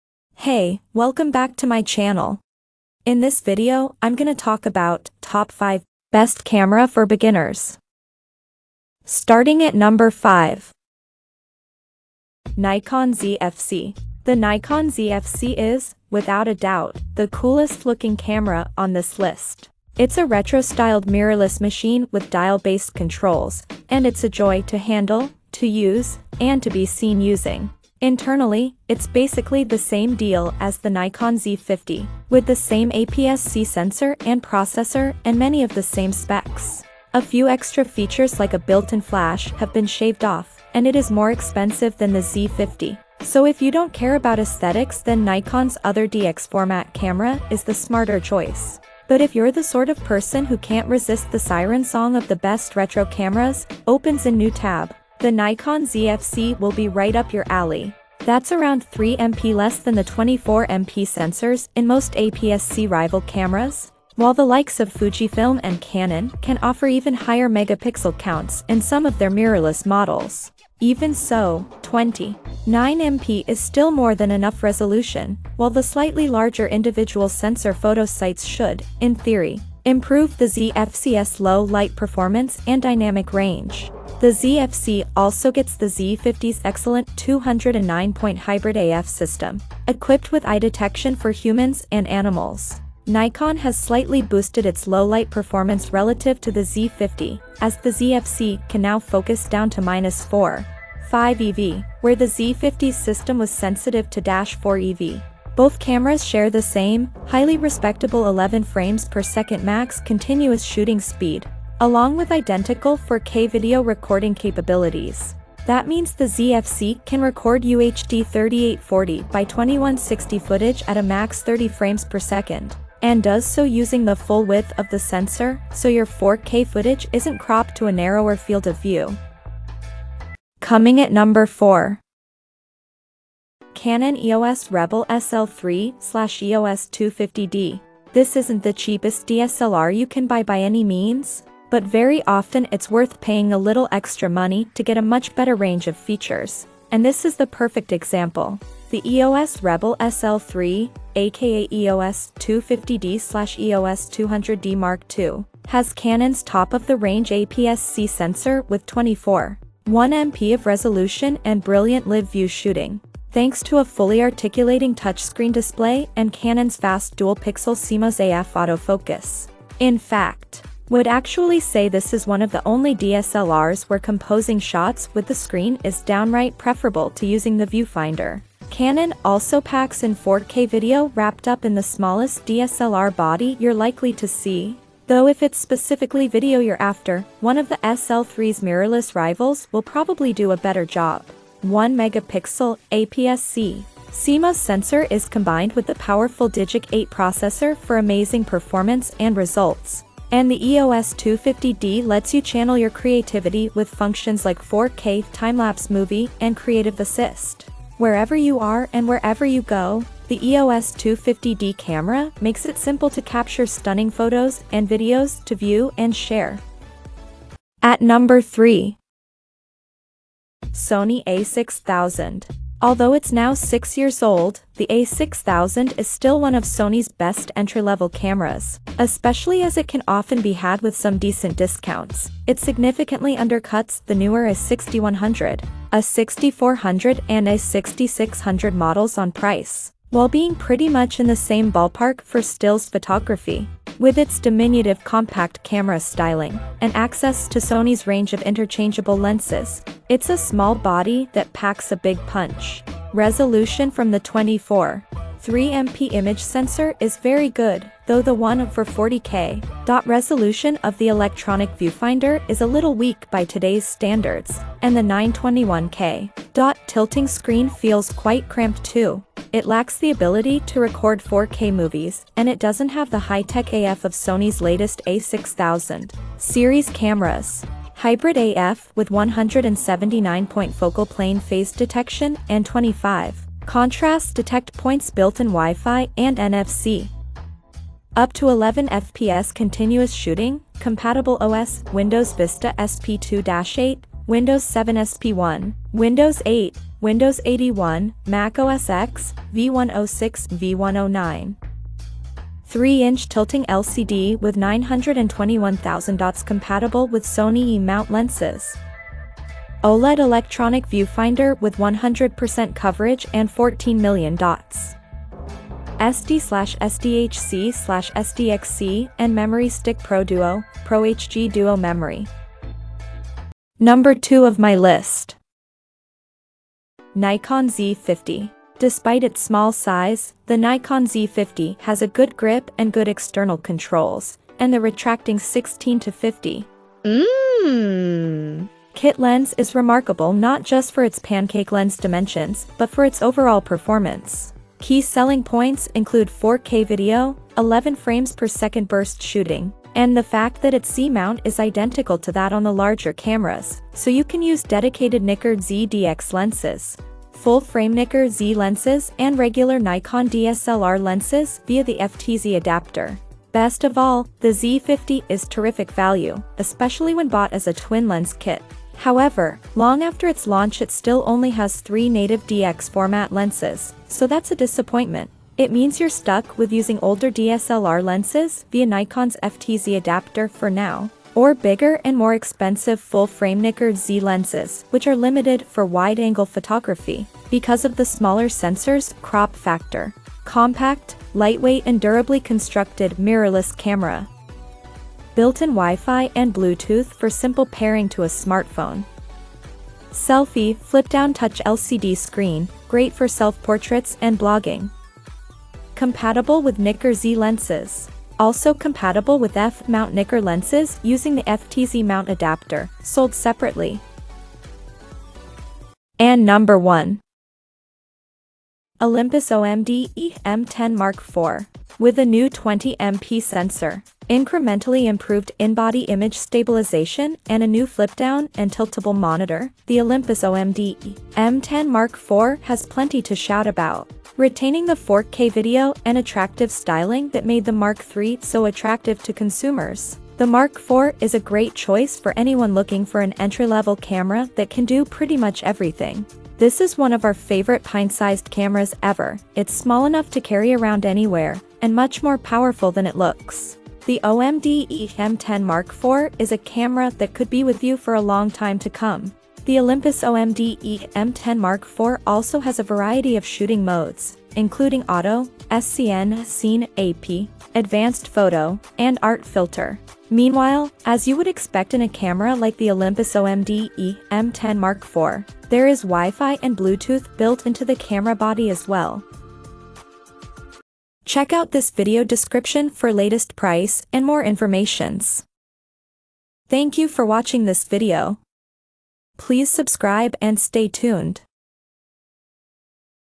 Top 5 Best Camera For Beginners 2022
► Here is the Updated Prices of Top 5 Best Camera For Beginners 2022!
1. Olympus OM-D E-M10 Mark IV

2. Nikon Z50

3. Canon EOS Rebel SL3 / EOS 250D

4. Sony A6000

5. Nikon Z fc